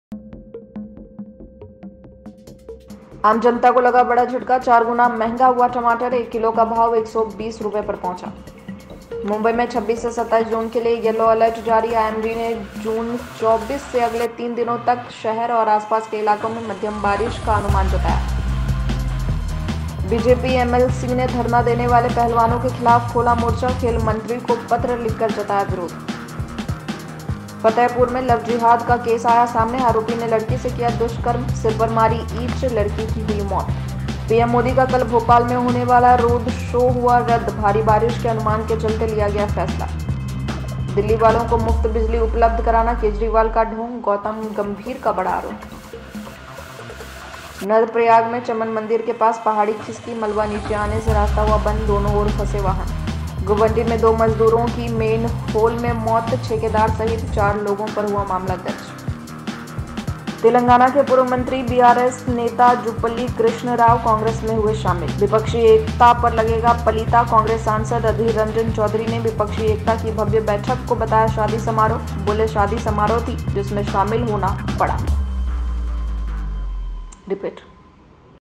JAGRUK TV : BULLETIN 26.06.2023
१-आम जनता को लगा बड़ा झटका, 4 गुना महंगा हुआ टमाटर, 1 किलो का भाव 120 रुपये पहुंचा

२-मुंबई - 26-27 जून के लिए येलो अलर्ट जारी,आईएमडी ने 24 जून से अगले तीन दिनों तक शहर और आसपास के इलाकों में मध्यम बारिश का पूर्वानुमान जारी किया है।

३-बीजेपी एमएलसी ने धरना देने वाले पहलवानों के खिलाफ खोला मोर्चा, खेल मंत्री को पत्र लिखकर जताया विरोध

४-फतेहपुर में लव जिहाद का केस, आरोपी ने लड़की से किया दुष्कर्म, सिर पर मारी ईंट, मौत

५-PM मोदी का कल भोपाल में होने वाला रोड शो रद्द, भारी बारिश के अनुमान के चलते लिया फैसला

६-दिल्लीवालों को मुफ्त बिजली उपलब्ध कराना केजरीवाल का ढोंग: गौतम गंभीर

७-नंदप्रयाग में चमन मंदिर के पास पहाड़ी खिसकी, मलबा नीचे आने से रास्ता बंद, दोनों ओर फंसे वाहन

८-गोवंडी में 2 मजदूरो की मैनहोल में मौत,ठेकेदार सहीत 4 लोगो पर मामला दर्ज

९-तेलंगाना के पूर्व मंत्री बीआरएस नेता जुपल्ली कृष्णा राव कांग्रेस में हुए शामिल

१०- विपक्षी एकता पर लगेगा पलीता! कांग्रेस सांसद अधीर रंजन चौधरी ने विपक्षी एकता की भव्य बैठक को बताया
-शादी समारोह  एक शादी समारोह थी, जिसमें कांग्रेस को शिष्टाचारवश शामिल होना पड़ा.